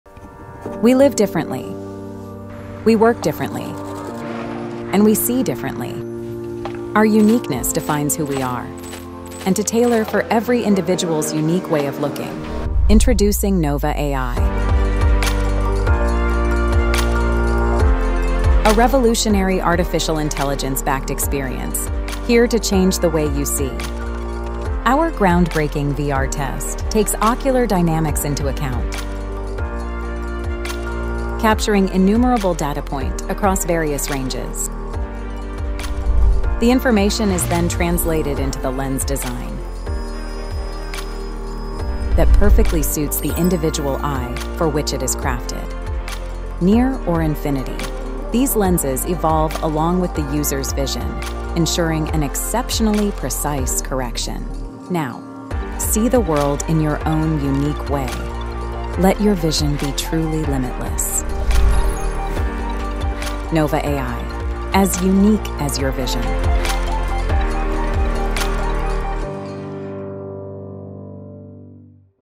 Experience cutting-edge progressive vision with Nova Ai lenses
Nova Ai is here to change the way you see. With the power of cutting-edge Artificial Intelligence, we capture and understand your unique eye movements and how you perceive your world. We then translate this data into lenses optimized for your eyes, ensuring precise vision in every scenario. Now, see the world your way.